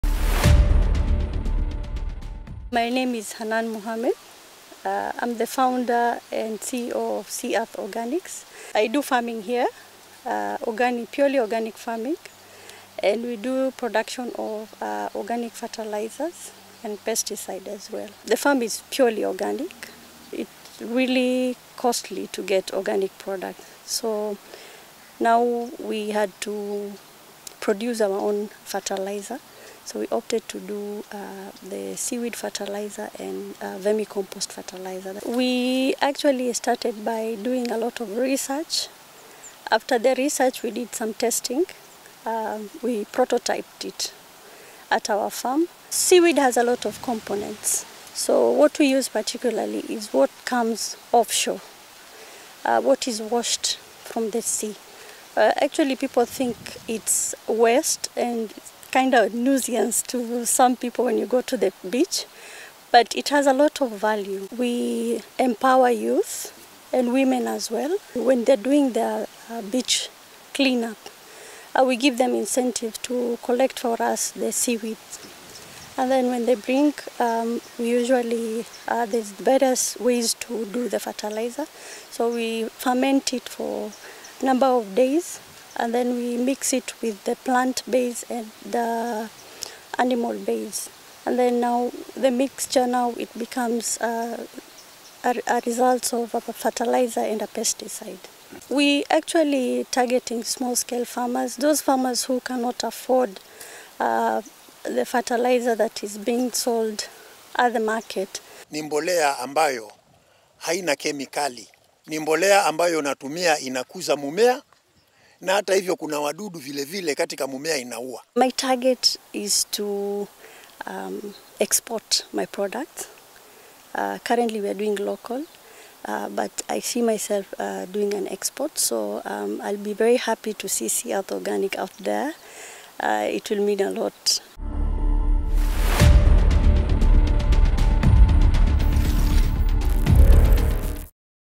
SME 101: How Kwale farmer is turning Indian Ocean seaweed into valuable fertiliser and pesticide
Hannan Mohamed, a farmer in Kwale County, researched and started making fertiliser and pesticides from seaweed washed off the Indian Ocean beaches and coastline. She reveals how her products have helped farmers within Msambweni area, and how she uses them on her passion fruits farm.